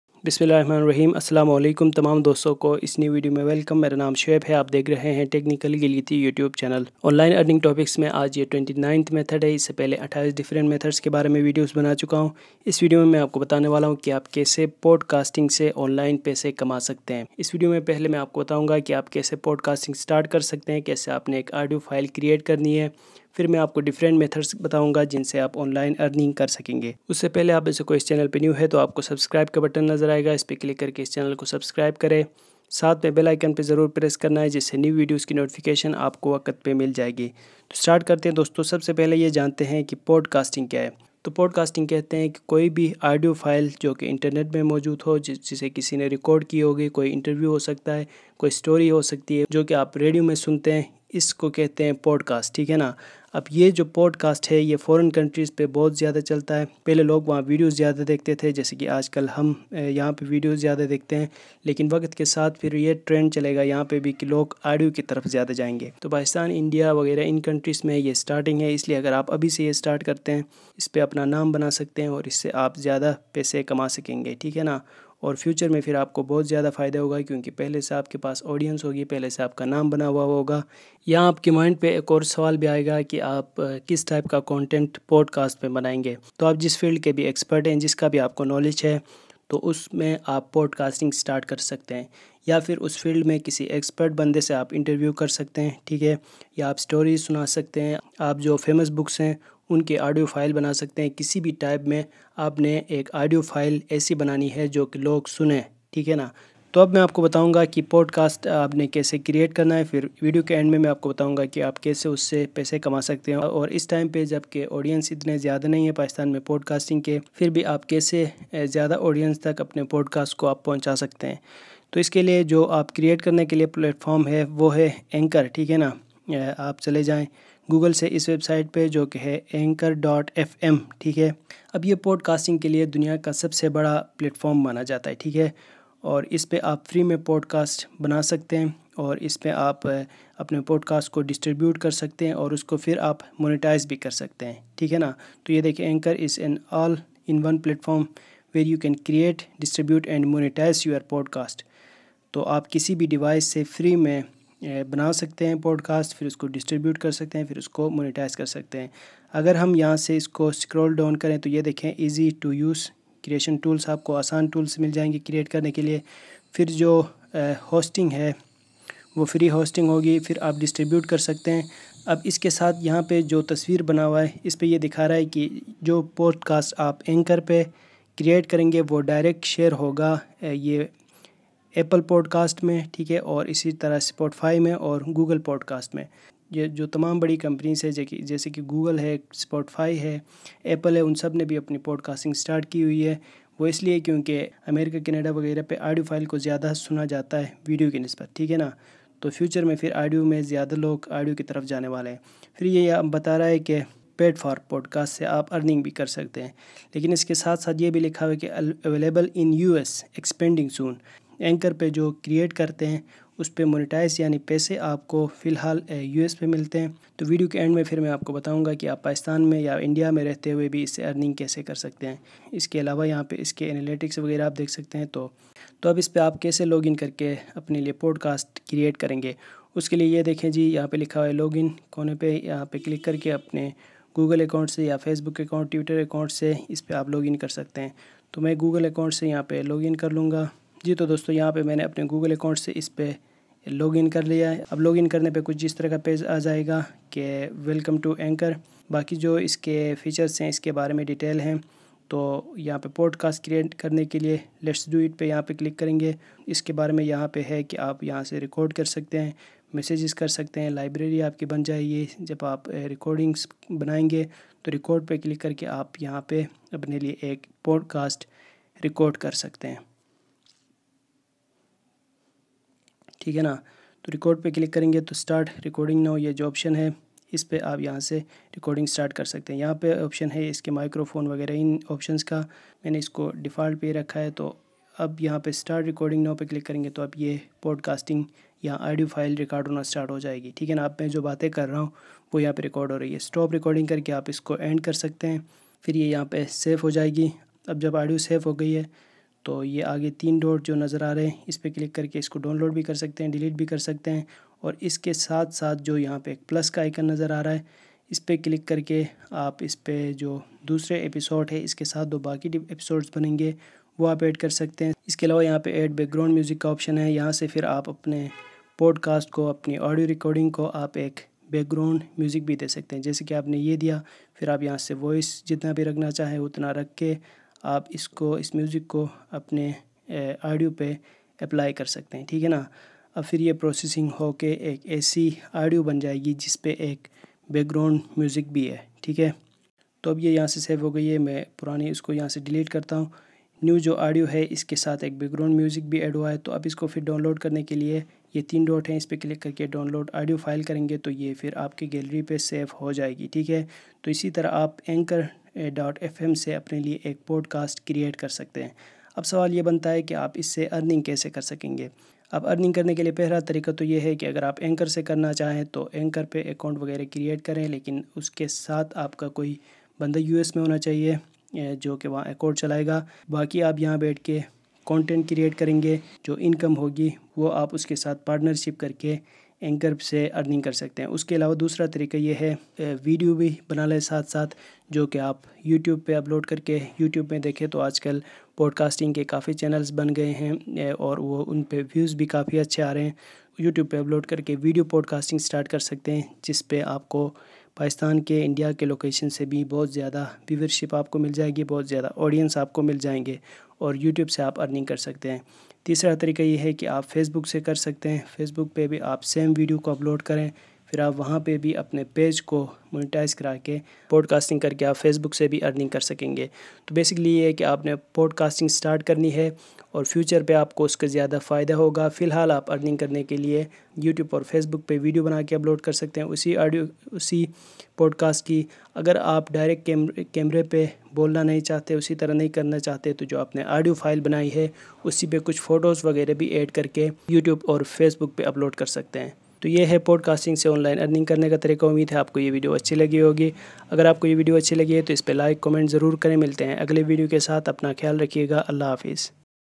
How to Start Podcast and Earn Money Online in Pakistan || Podcasting Kaise Start Kare in Urdu
Podcasting Kaise Start Kare in Urdu || How to Create Podcasts to Make Money Online | By Technical Gilgity in Urdu

29th Way To Earn Money Online
Podcast
How to Start Podcasting To Earn Money Online in Pakistan